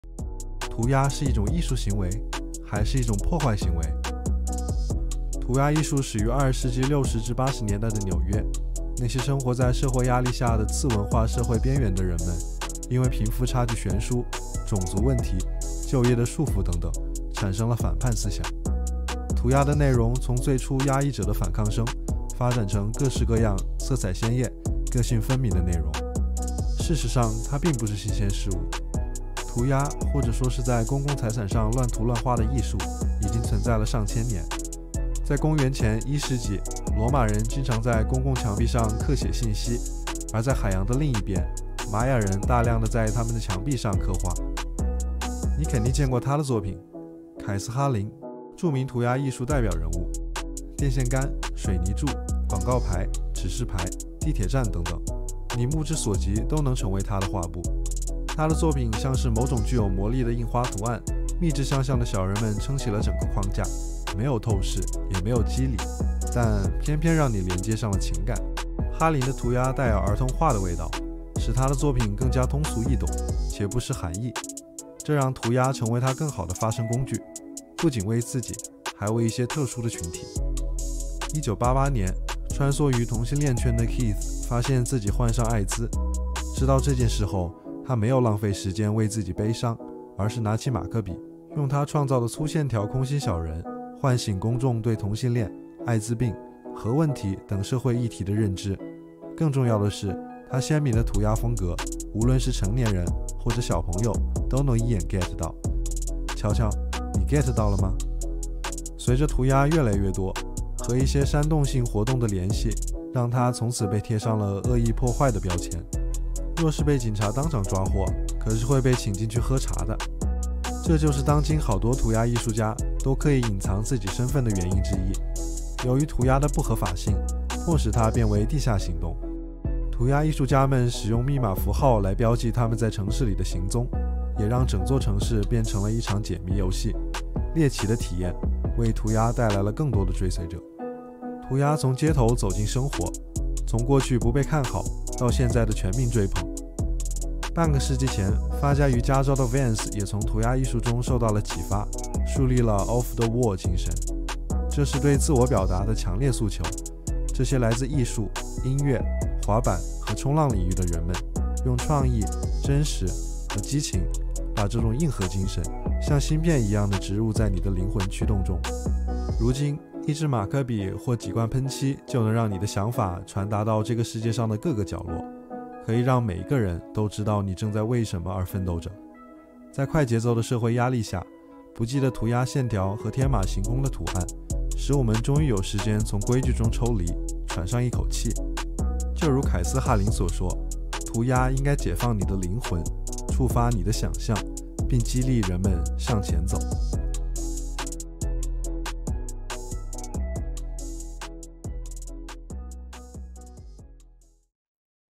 【dinlab】Graffiti Art - A guild to jail ｜ 是什么艺术，成功地引起了警察叔叔的注意？
Is Graffiti an art?
Or vandalism?

"Art should be something that liberates the soul, provokes the imagination and encourages people to go further." — Keith Haring

涂鸦艺术，你的吃牢饭指南
涂鸦是一种艺术行为？还是一种破坏行为？为什么充满危险又经久不衰？
“涂鸦应该解放你的灵魂，触发你的想像，并激励人们向前走。”——凯斯 · 哈林

🪐Want to know more about art design? follow our dinlab universe radio, Like, Favorite, and we will be sharing next clip. See you soon~
这里是dinlab宇宙电台，下一个视频与你继续分享

Reference 03:42